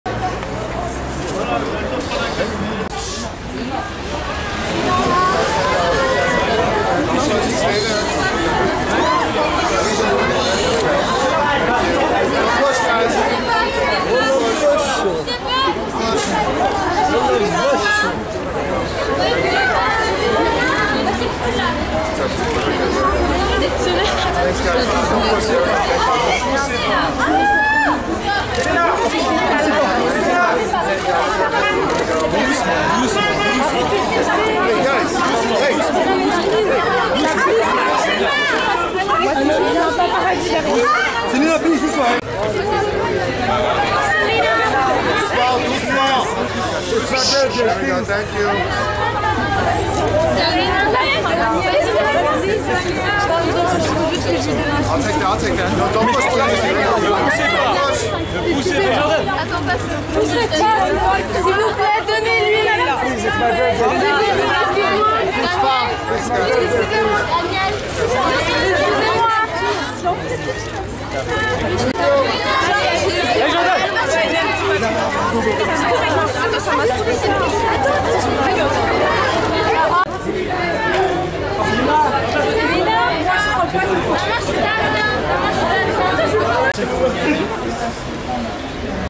VIDEO Selena GOMEZ with fans @ Paris 10 march 2016 - 10 mars
Off the shoulder dress for Selena GOMEZ with her fans outside of the Royal Monceau hotel @ Paris thursday 10 march 2016 - jeudi 10 mars - stars à Paris en Rencontre et dédicace